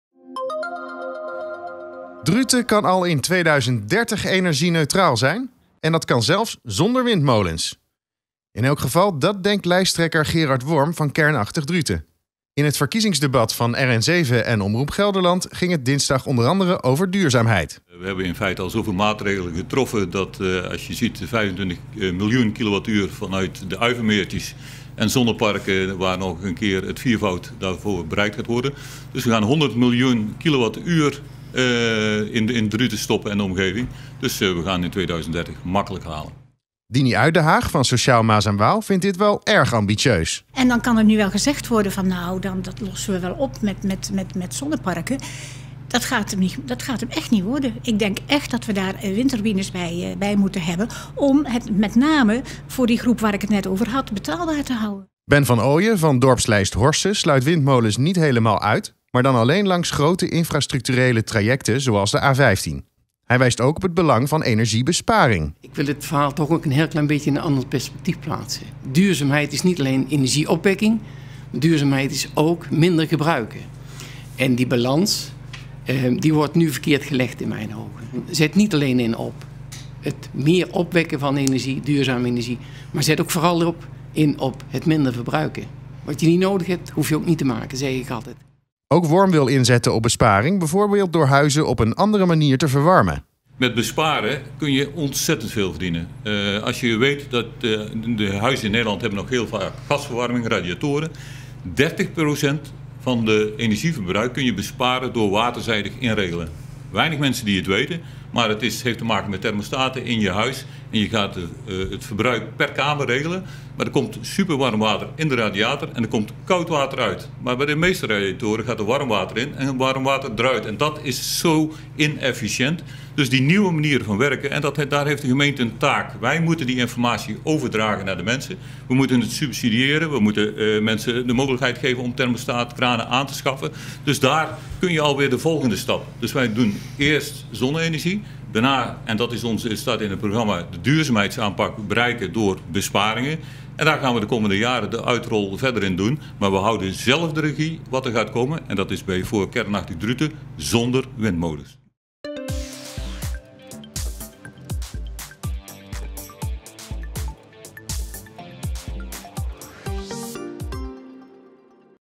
Zijn windmolens in Druten nodig om energiedoelstelling te halen?
DRUTEN - Over acht jaar kan de gemeente Druten al energieneutraal zijn, en daarvoor zijn geen windmolens nodig. Dat denkt althans lijsttrekker Gerard Worm van Kernachtig Druten. In het verkiezingsdebat dat RN7 en Omroep Gelderland dinsdag organiseerden, gingen de partijen onder andere in discussie over de mogelijke bouw van windturbines.